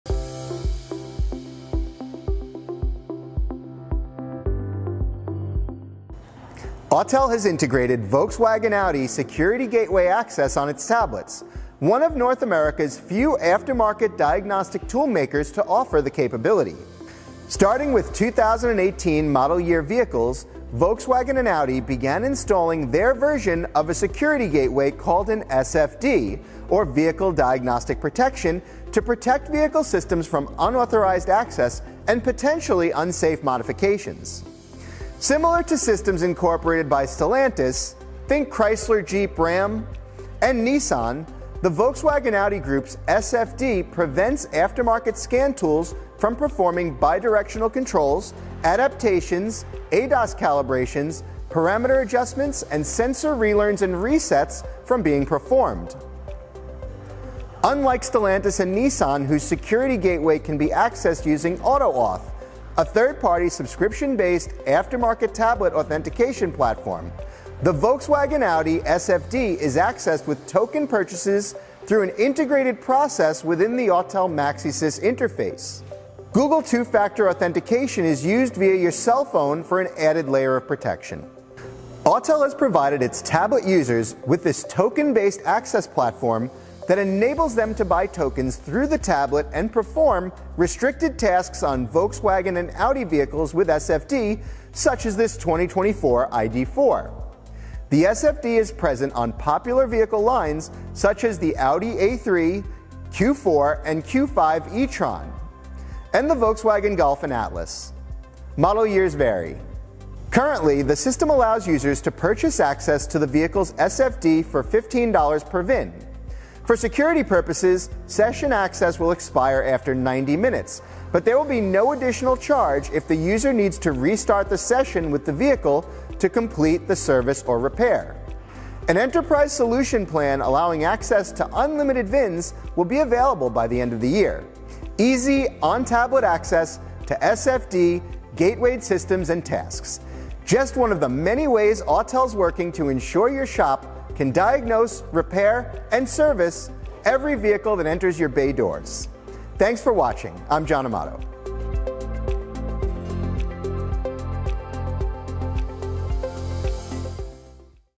Access VW & Audi Security Gateway on Autel Tablets
Autel is proud to be one of North America's few aftermarket diagnostic tool makers to offer Volkswagen/Audi Security Gateway (SFD) Access directly on our tablets.

Starting with 2018 models, Volkswagen and Audi introduced a system designed to protect vehicles from unauthorized access. This security gateway prevents bi-directional controls, ADAS calibrations, parameter adjustments, and more without proper authentication. With Autel's MaxiSYS tablets, you can now access Volkswagen/Audi SFD using an integrated token-based system.

This feature empowers your shop to perform restricted tasks, including ADAS calibrations, sensor resets, and parameter adjustments, ensuring you can diagnose, repair, and service every vehicle that comes through your bay doors.

Supported Models: Popular vehicles like the Audi A3, Q4, Q5 Etron, Volkswagen Golf, and Atlas.